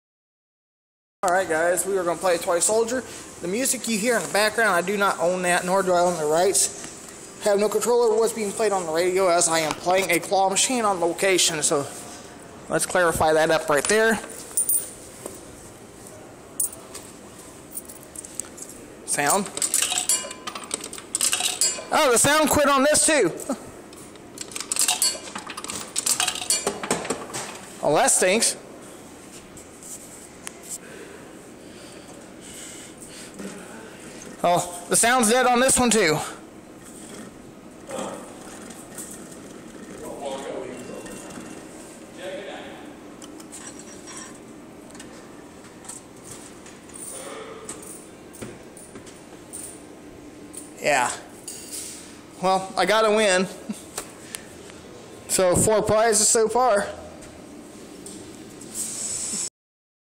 Toy Soldier Claw Machine @ Gophers Grill sound is dead
today i head over to my local gophers grill to face the toy soldier claw machine it is the 1 dollar claw   its a 50 cent size claw machine that is 1 dollar per play  sound was dead on the machine